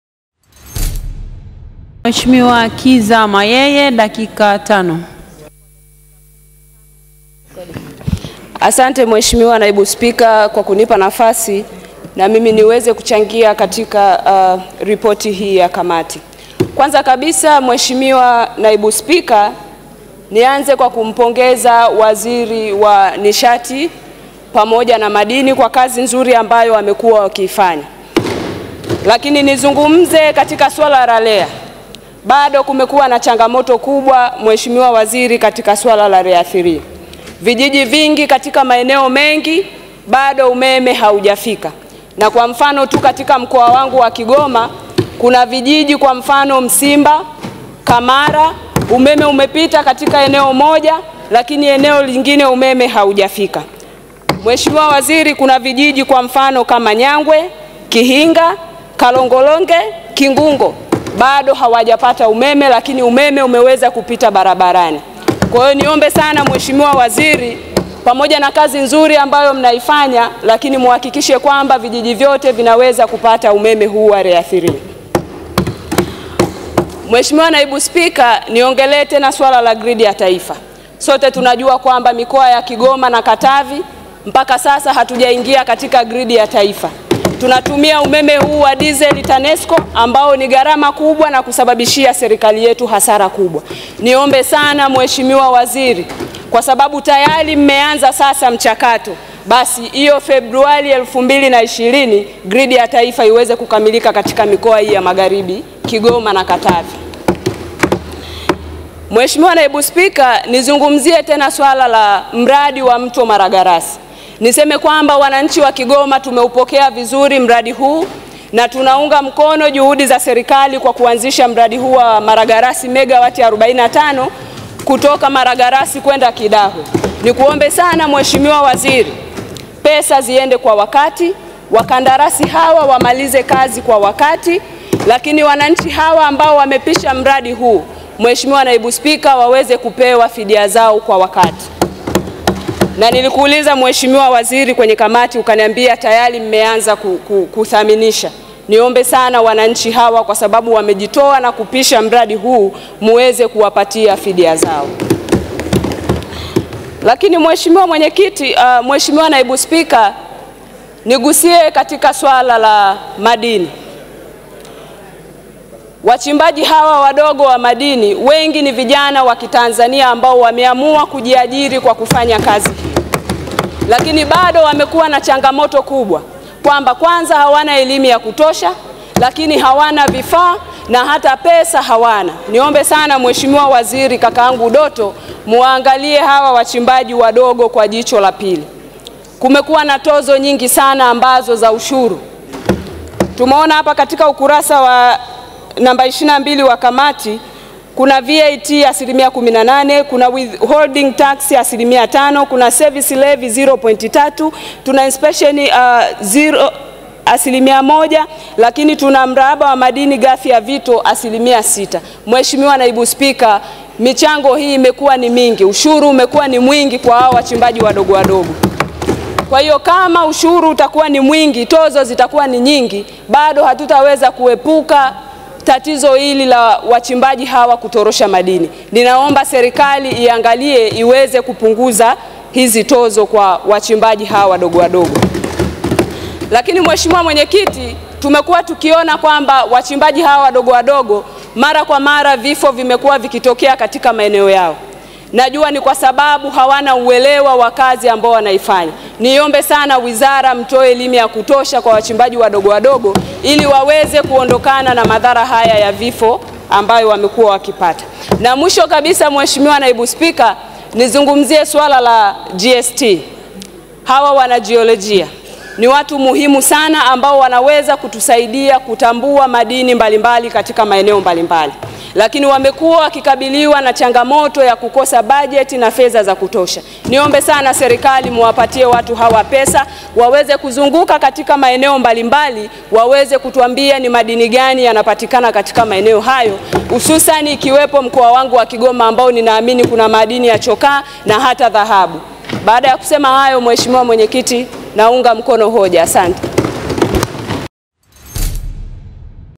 Ushauri wa Mbunge Kiza Mayeye kwa Serikali ya JPM
Mbunge wa viti maalum kwa tiketi ya CUF Kiza Mayeye ni miongoni mwa Wabunge walioweza kusimama Bungeni Dodoma leo February 6, 2019 ili kuchangia mapendekezo yake katika kujadili hoja mbili za kamati ikiwemo kamati ya Bunge ya mambo ya nje, Ulinzi na usalama mwaka 2018 pamoja na kamati ya Nishati na madini